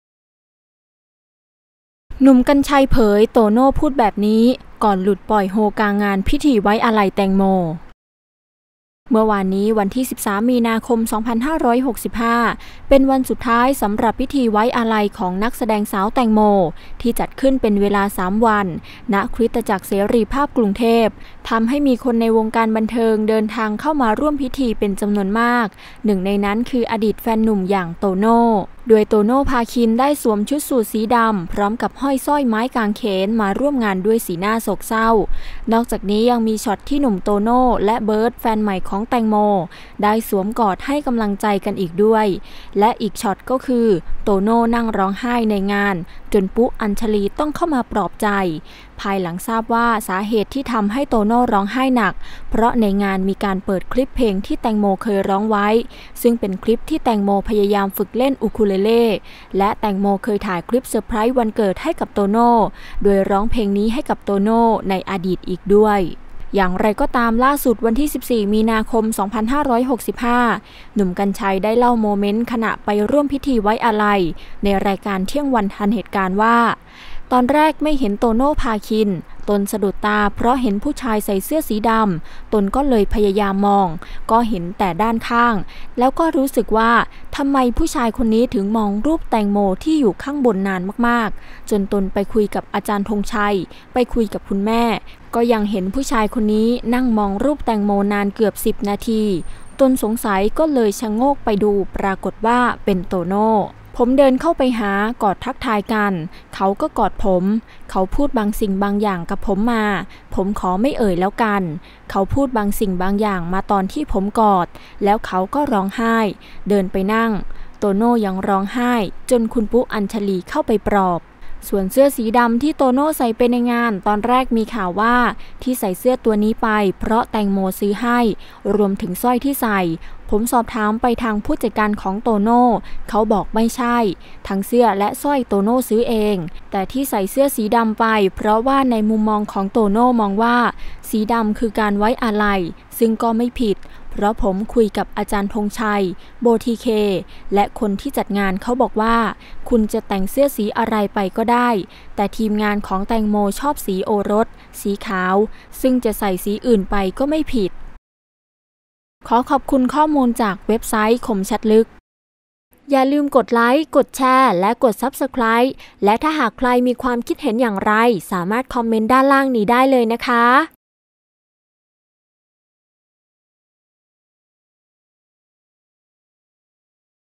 หนุ่ม กรรชัย มาแล้ว เผยความจริงเกี่ยวกับ โตโน่ พร้อมคำพูดที่ทำเอาร้องไห้
อย่างไรก็ตามล่าสุด (14 มีนาคม 2565) "หนุ่ม กรรชัย" ได้เล่าโมเมนต์ขณะไปร่วมพิธีไว้อาลัย ในรายการเที่ยงวันทันเหตุการณ์ว่า ตอนแรกไม่เห็น "โตโน่ ภาคิน" ตนสะดุดตาเพราะเห็นผู้ชายใส่เสื้อสีดำ ตนก็เลยพยายามมองก็เห็นแต่ด้านข้าง แล้วก็รู้สึกว่าทำไมผู้ชายคนนี้ถึงมองรูปแตงโมที่อยู่ข้างบนนานมาก จนตนไปคุยกับ อ.ธงชัย ไปคุยกับคุณแม่ ก็ยังเห็นผู้ชายคนนี้นั่งมองแตงโมนานมากเกือบ 10 นาที ตนสงสัยก็เลยชะโงกไปดูปรากฏว่าเป็นโตโน่